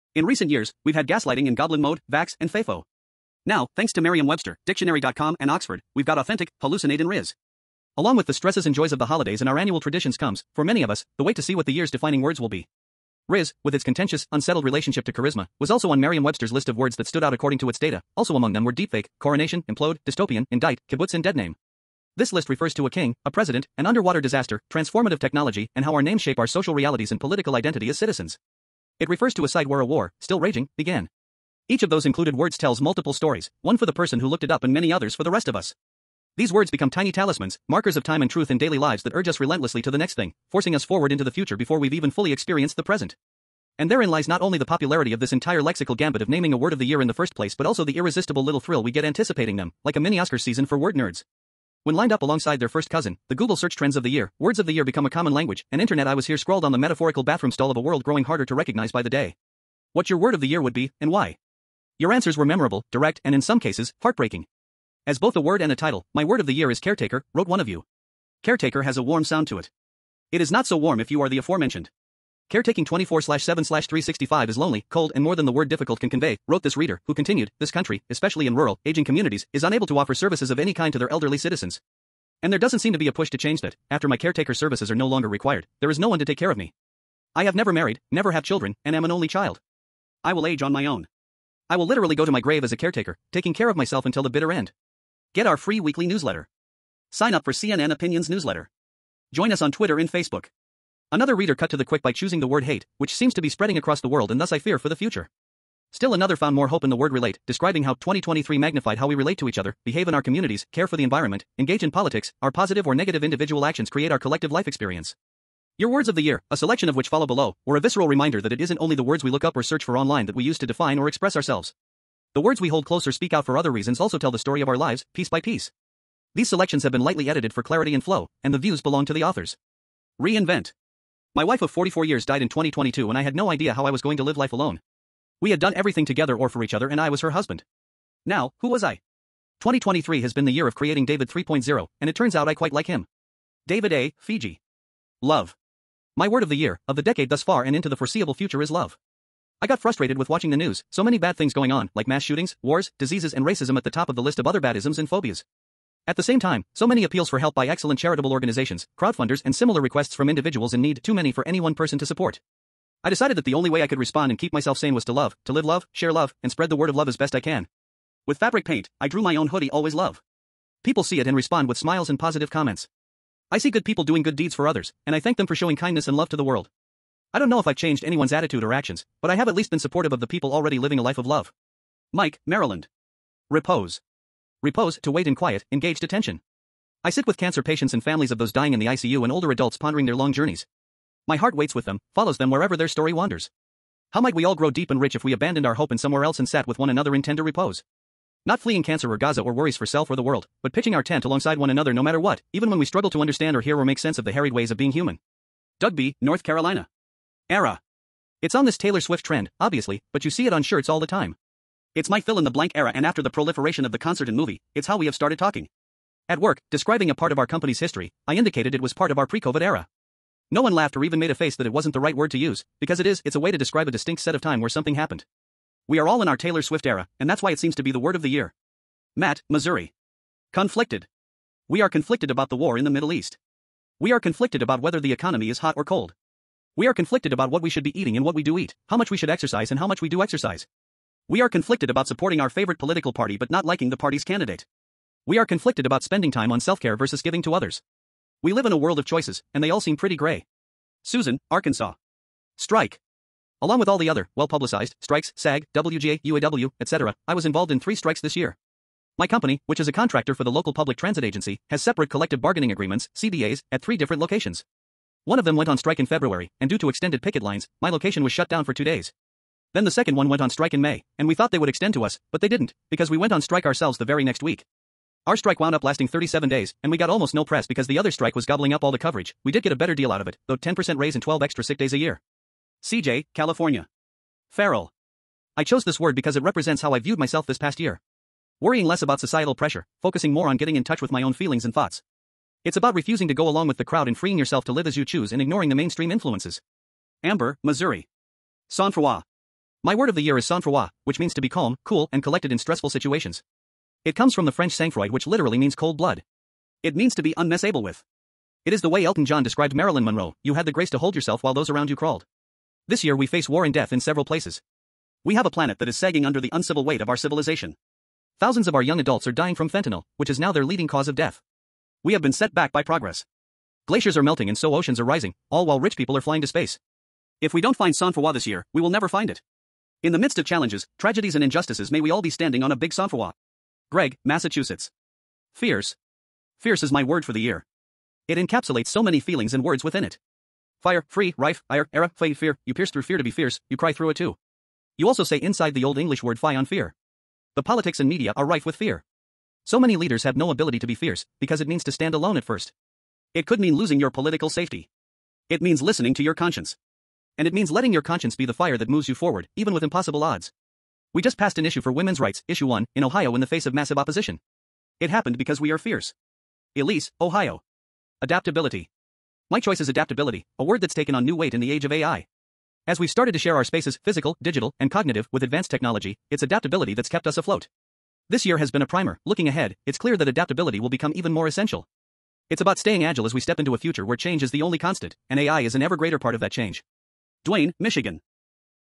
Dictionaries have shared their word of the year. We wanted to know yours
Rizz (with its contentious, unsettled relationship to charisma) was also on Merriam-Webster’s list of words that stood out according to its data; also among them were deepfake, coronation, implode, dystopian, indict, kibbutz and deadname.

This list refers to a king, a president, an underwater disaster, transformative technology and how our names shape our social realities and political identity as citizens. It refers to a site where a war, still raging, began. Each of those included words tells multiple stories – one for the person who looked it up and many others for the rest of us.